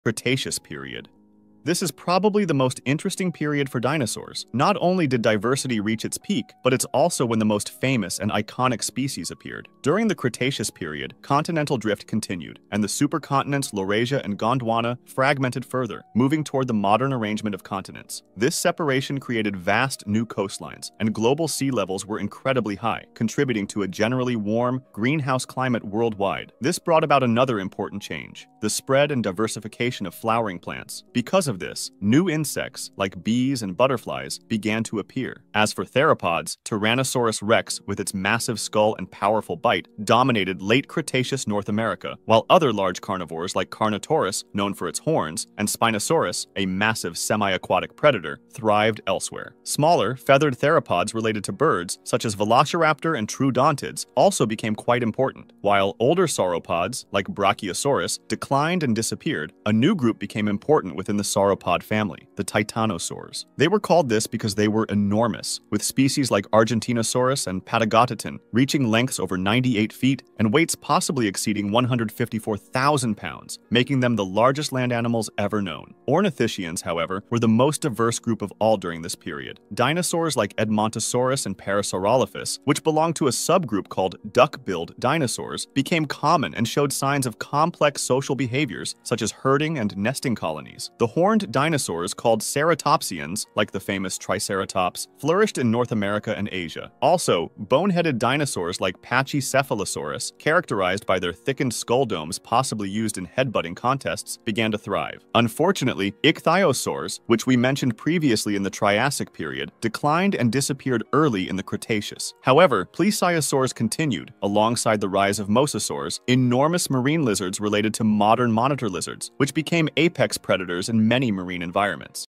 🦖 Cretaceous Giants: The Dinosaurs' Final Reign
Explore the Cretaceous period's peak dinosaur diversity, from T-Rex to titanosaurs. Continental drift, warm climates, and flowering plants shaped this iconic era. Discover the rise of new species and behaviors!

Video created with the support of AI. Original concept by The Paint Explainer. Curated and Edited by Explain It Simply.